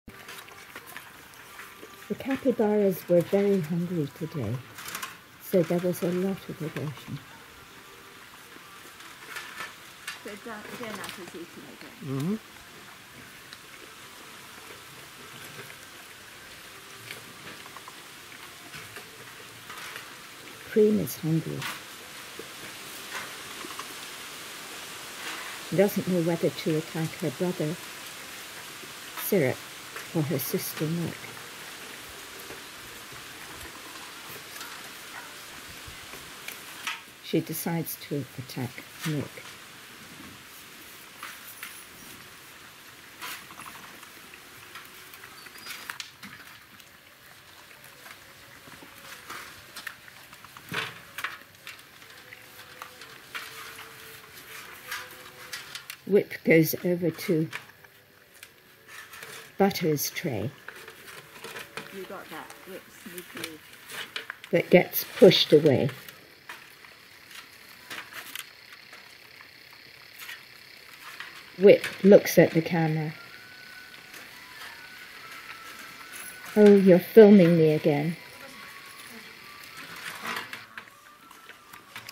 Capybara Dinner Routine Starring Cream, Milk, Syrup, Whip, Butter and Donut
Cream can't decide who to attack, her brother, Syrup or her sister, Milk. Whip meekly gives way to Butter. Donut eats soil, as he so often does. There must be minerals in the soil that he needs. I don't see many of the other capybaras eating soil, but the youngest generations were Zabon's babies and she was too weak to teach them.

WN VO Cream decides to intimidate Milk not Syrup Donut soil 18 Jan 2023 025